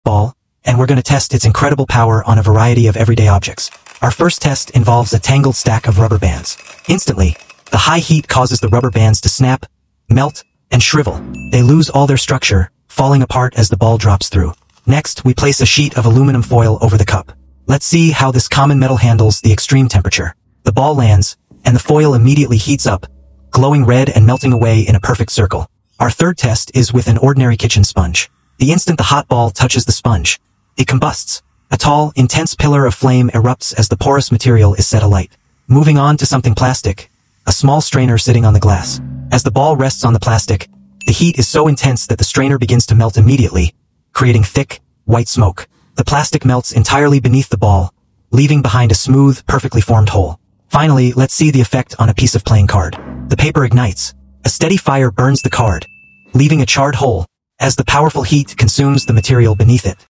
​🤯 Watch a Glowing Hot Ball Melt Through EVERYTHING! (Science Experiment)
​🔥 The Unstoppable Heat Ball: What Happens Next Will Shock You!
​Ever wondered what a glowing hot metal ball can do? We put this intensely heated sphere to the ultimate test against everyday objects! Watch as it slices through rubber bands, melts aluminum foil, ignites a sponge, and burns a hole straight through a playing card.
​This is a super satisfying and visually stunning science demonstration of extreme heat and energy transfer. The power of thermodynamics is incredible!